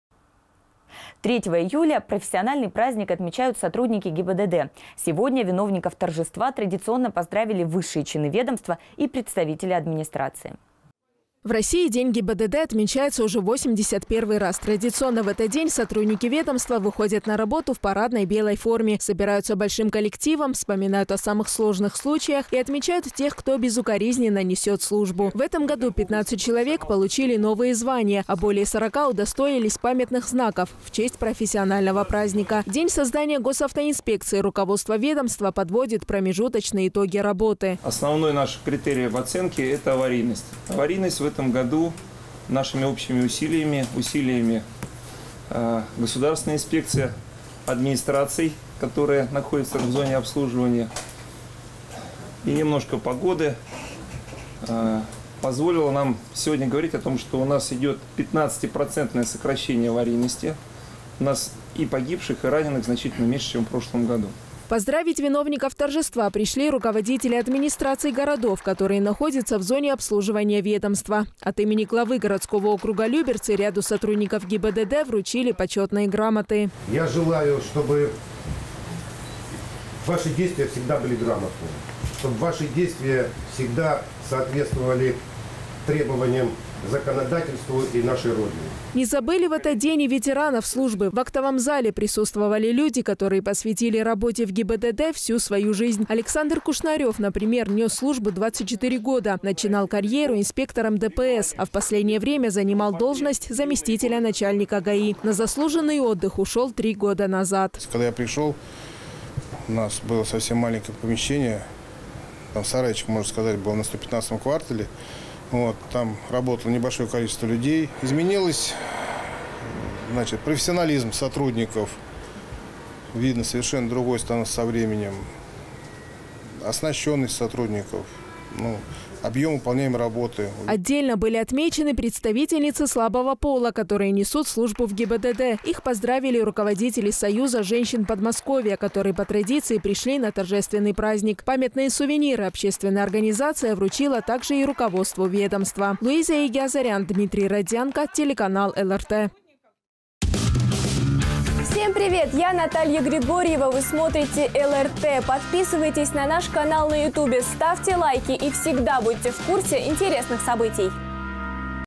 Сотрудников ГИБДД поздравили с профессиональным праздником
3 июля профессиональный праздник отмечают сотрудники ГИБДД. Виновников торжества традиционно поздравили высшие чины ведомства, а также представители администрации.

В России день ГИБДД отмечается уже 81 раз. Традиционно, в этот день сотрудники ведомства выходят на работу в парадной белой форме, собираются большим коллективом, вспоминают о самых сложных случаях и отмечают тех, кто безукоризненно несет службу. В этом году 15 человек получили новые звания, а более 40 удостоились памятных знаков в честь профессионального праздника. В день создания Госавтоинспекции, руководство ведомства подводит промежуточные итоги работы.

Илья Атаманов, начальник отдела ГИБДД МУ МВД России «Люберецкое»:
"Основной наш критерий в оценке – это аварийность. Аварийность в этом году нашими общими усилиями, усилиями Государственной инспекции, администраций, которые находятся в зоне обслуживания, сокращена на 15%. И погибших и раненных значительно меньше, чем в прошлом году".

Поздравить виновников торжества пришли руководители администраций городов, которые  находятся в зоне обслуживания ведомства. От имени главы городского округа Люберцы ряду сотрудников ГИБДД вручили почетные грамоты.

Эдуард Сигалов, заместитель руководителя администрации городского округа Люберцы:
"Я желаю, чтобы ваши действия всегда были грамотные, чтобы ваши действия всегда соответствовали требованиям законодательства и нашей Родины".

Не забыли в этот день и ветеранов службы. В актовом зале присутствовали люди, которые посвятили работе в ГИБДД всю свою жизнь. Александр Кушнарев, например, нес службу 24 года, начинал карьеру инспектором ДПС, а в последнее время занимал должность заместителя начальника ГАИ. На заслуженный отдых ушел три года назад.

Александр Кушнарев, ветеран Люберецкого ОГИБДД:
"Когда я пришел, у нас было совсем маленькое помещение, сарайчик, можно сказать, на 115 квартале, там работало небольшое количество людей. Изменился профессионализм сотрудников, видно, совершенно другой стал со временем, оснащенность сотрудников, объем выполняемой работы увеличился".

Отдельно были отмечены представительницы слабого пола, которые несут службу в ГИБДД. Их поздравили руководители «Союза женщин Подмосковья», которые, по традиции, пришли на торжественный праздник. Памятные сувениры общественная организация вручила также руководству ведомства.